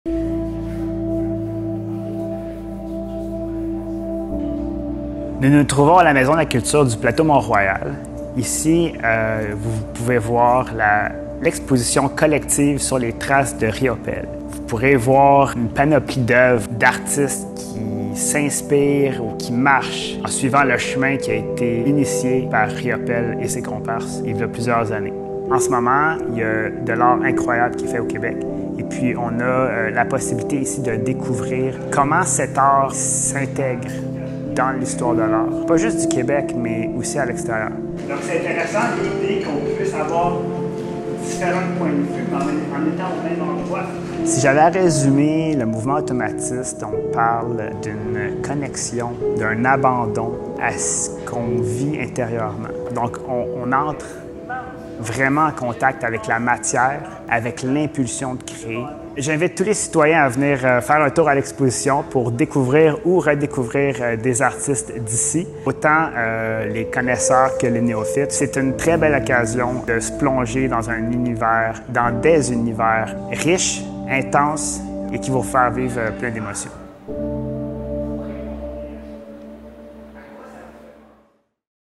Visite guidée de l'exposition hommage à Jean Paul Riopelle
Visitez virtuellement l'exposition "Sur les traces des Automatistes" en compagnie d'Emmanuel Laflamme, artiste et médiateur culturel. Des visites guidées et des ateliers en lien avec cette exposition auront lieu tout au long de l'été en sa compagnie et sont offerts gratuitement aux groupes et aux citoyens et citoyennes, sur inscription.

Jusqu'au 13 août, vous pouvez aussi accéder gratuitement à l’exposition dans la grande salle de la maison de la culture (465, avenue du Mont-Royal Est) du mardi au jeudi de 13 h à 19 h et du vendredi au dimanche de 13 h à 17 h.

(Vidéo : Henri Gionet)

Avec les œuvres de Jean Paul Riopelle, Françoise Sullivan, Armand Vaillancourt, André Fournelle, Mathieu Laca, Bertrand Lavoie, PDG - Michel Pedneault, Bob Desautels, Pierrot Gaudreau, Dominique Desbiens, la poète Claudine Bertrand, et Julie Robinson, artiste et commissaire.